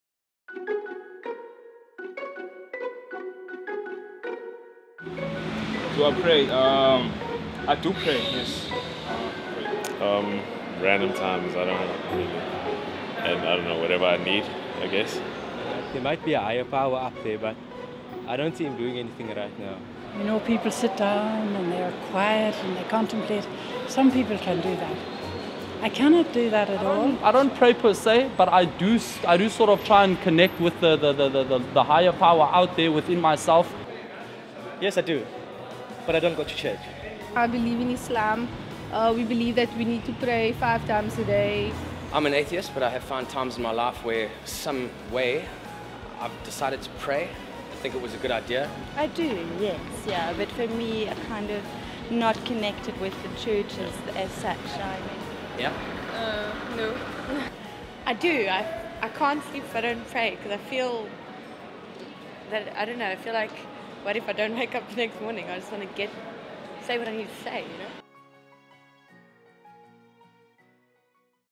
Alpha Vox Pops - How and Why Should We Pray?
We asked Capetonians if and why they prayed.